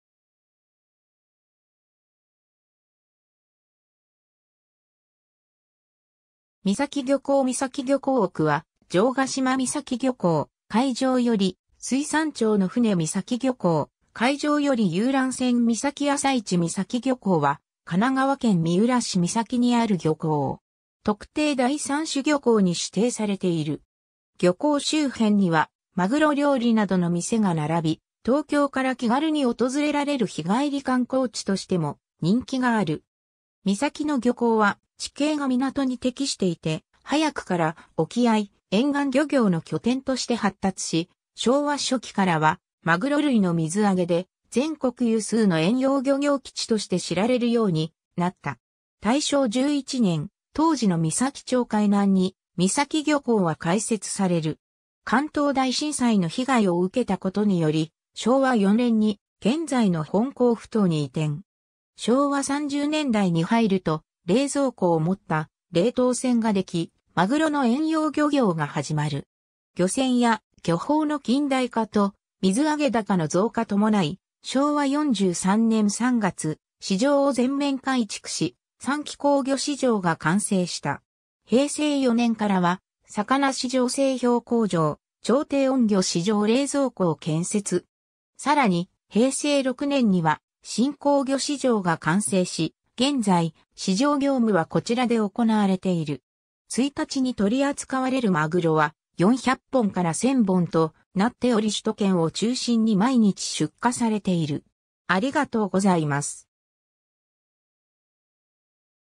三崎漁港
三崎漁港 三崎漁港　奥は城ヶ島 三崎漁港・海上より水産庁の船 三崎漁港・海上より遊覧船（にじいろさかな号） 三崎朝市 三崎漁港（みさきぎょこう）は、神奈川県三浦市三崎にある漁港。
特定第3種漁港に指定されている。
 漁港周辺には、マグロ料理などの店が並び、東京から気軽に訪れられる日帰り観光地としても人気がある。
 三崎の漁港は、地形が港に適していて、早くから沖合・沿岸漁業の拠点として発達し、昭和初期からはまぐろ類の水揚げで全国有数の遠洋漁業基地として知られるようになった。
 大正11年、当時の三崎町海南（現在の三崎公園付近）に三崎漁港は開設される。
関東大震災の被害を受けたことにより、昭和4年に現在の本港埠頭に移転。
昭和30年代に入ると、冷蔵庫を持った冷凍船が出来、マグロの遠洋漁業がはじまる。
漁船や漁法の近代化と水揚高の増加伴い、昭和43年3月、市場を全面改築し、三崎港魚市場が完成した。
 平成4年からは、魚市場製氷工場、超低温魚市場冷蔵庫を建設。
さらに、平成6年には新港魚市場が完成し、現在、市場業務はこちらで行われている。
1日に取扱われるマグロは、400本から1,000本となっており首都圏を中心に毎日出荷されている。